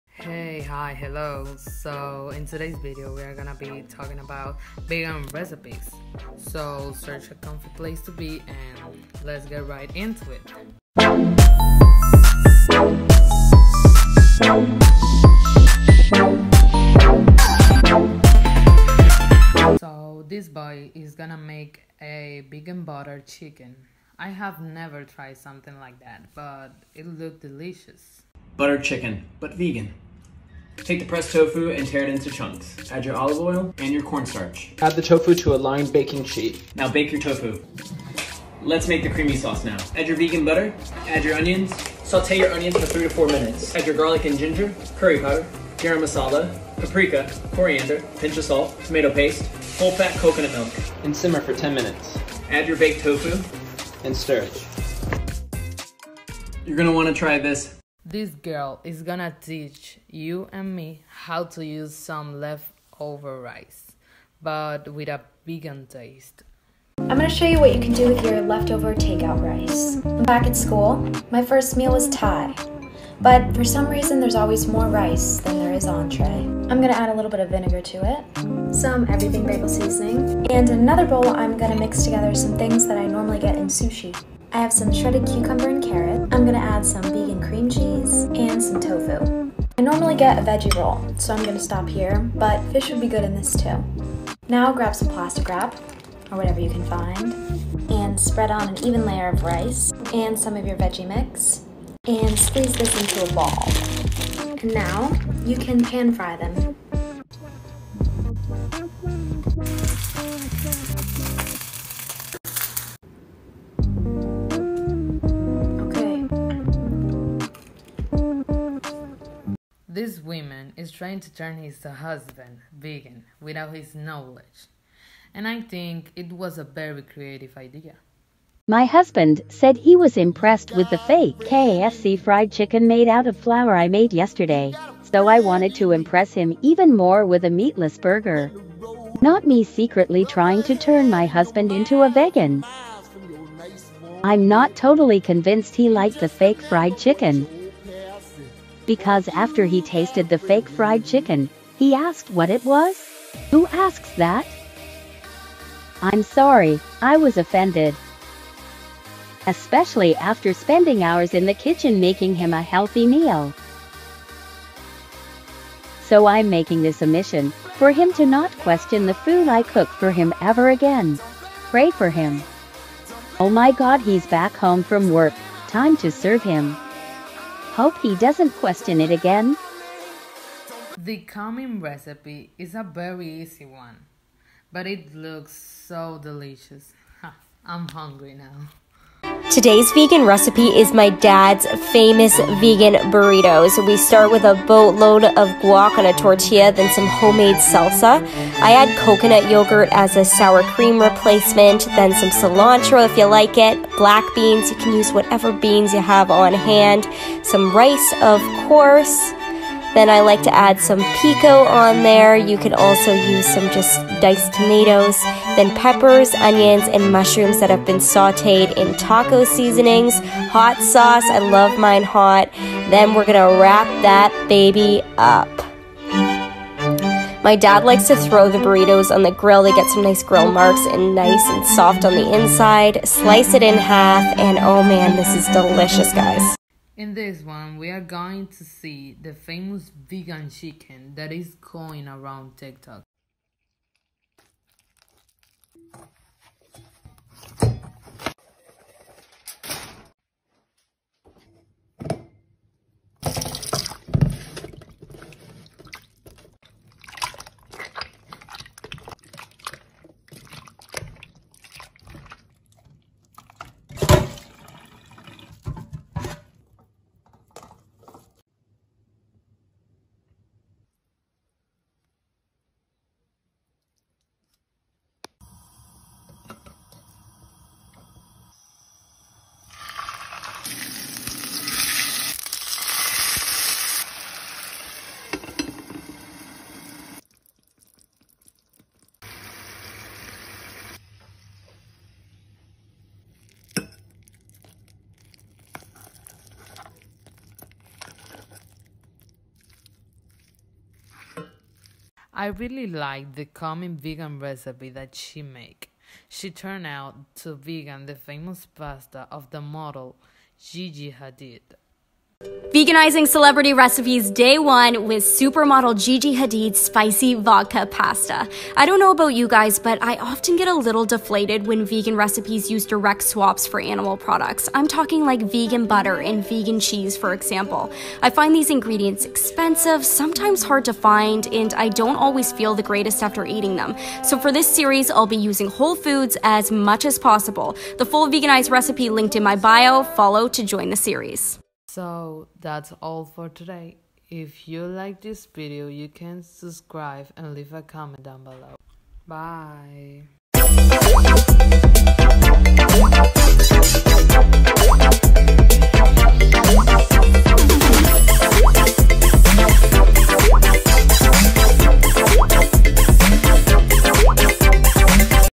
vegan recipes | tik tok compilation
so in today’s video in gonna be giving my opinion in some vegan recipes, i hope y’all enjoy it ;)